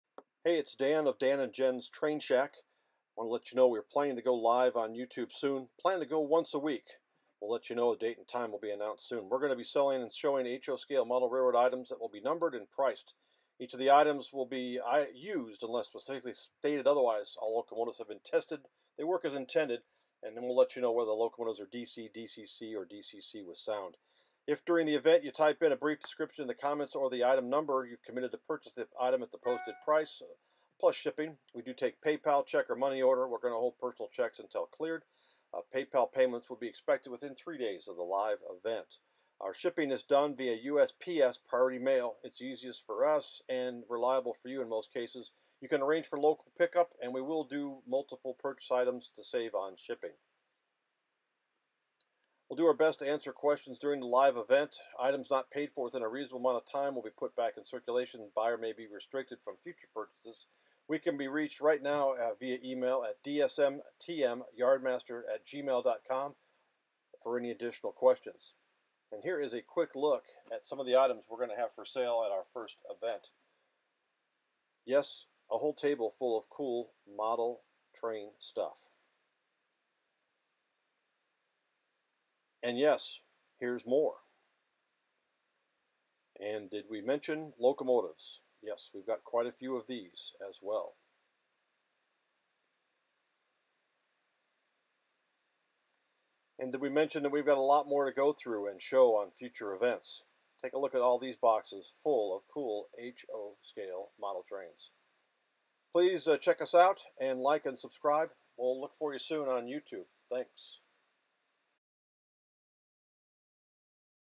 Dan & Jen's Train Shack 2
This video explains how we'll be selling model trains on Youtube.